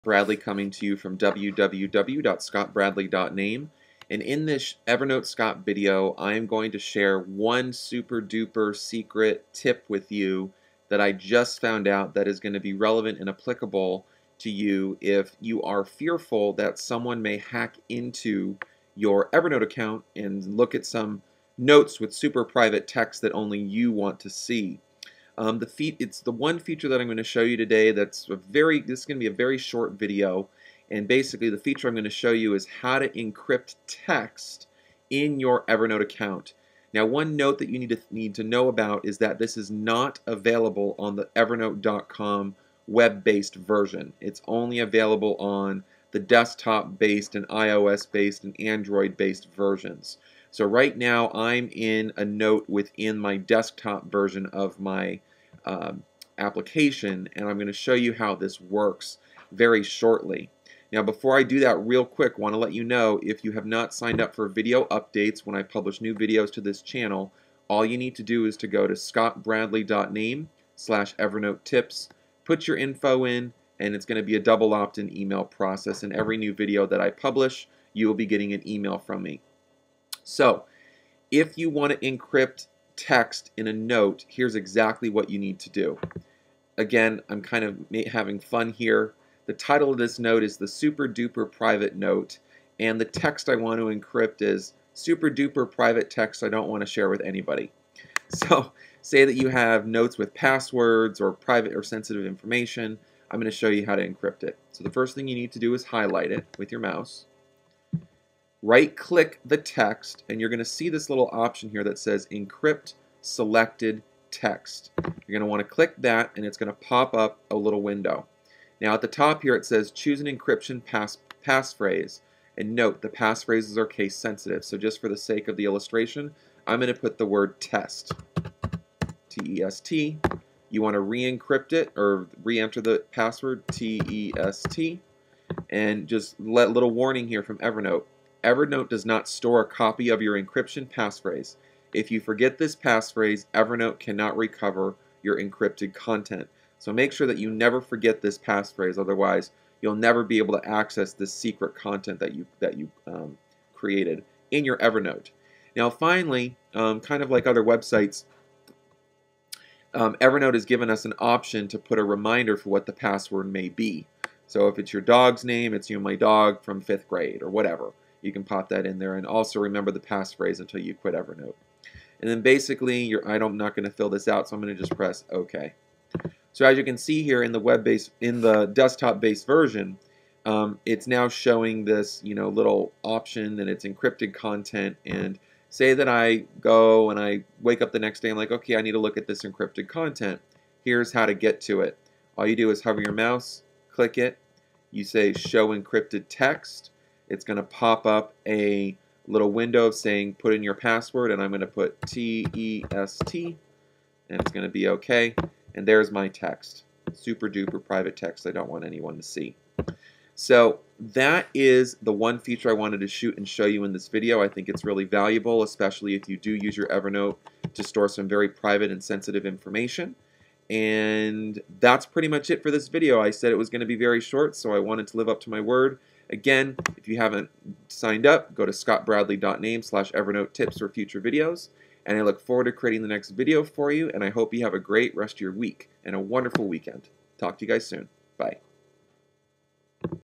Evernote Tips: How To Encrypt Text in Evernote
- In this Evernote video I am going to share with you how to encrypt text in any note that you create within an Evernote notebook. This feature comes in handy, especially when you want to keep sensitive information stored in your Evernote account!

Video #1: The 11 Amazing Features That Make Using Evernote So Freaking Awesome